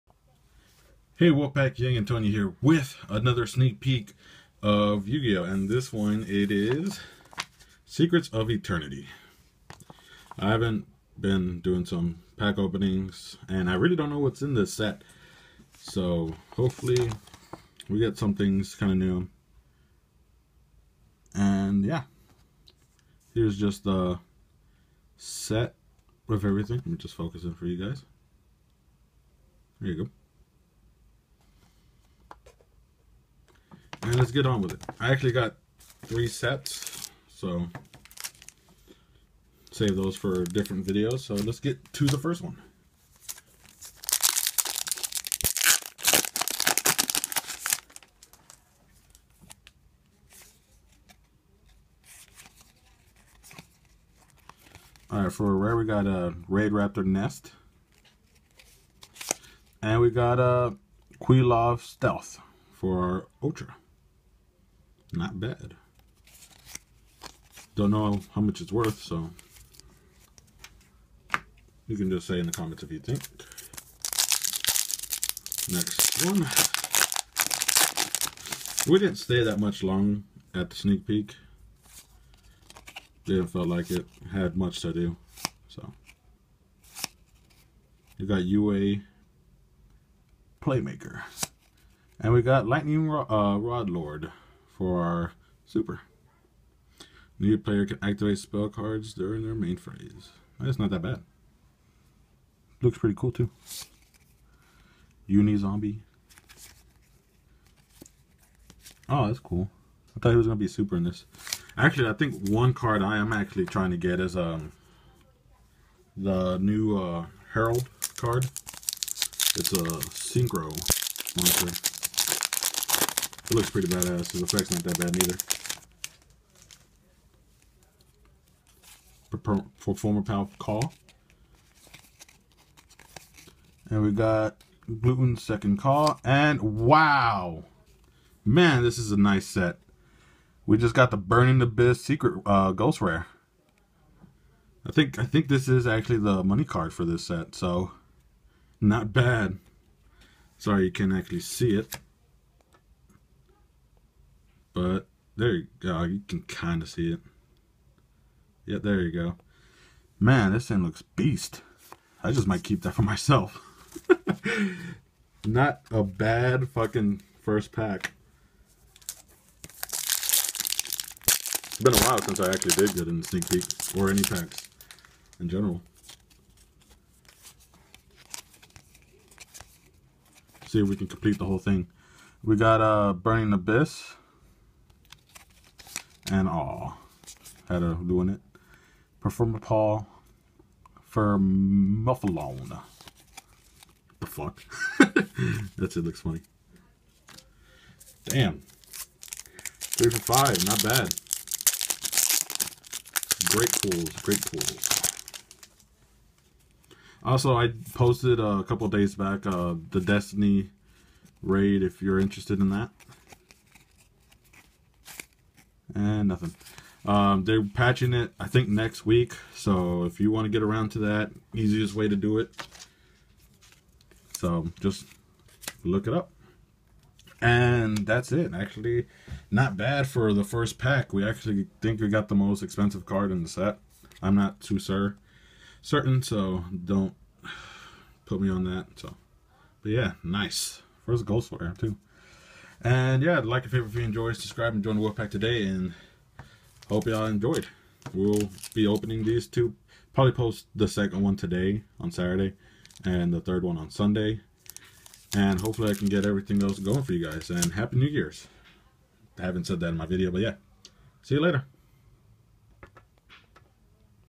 Yu-Gi-Oh! Sneak Peek Secrets of Eternity AMAZING PULLS!!!
Hey Wolf Pack, Yang Antonio here with Secrets of Eternity Sneak Peek and man did we get amazing pulls from this set so hope you enjoy and hit that like button and subscribe and join the wolf pack today for other amazing videos!!!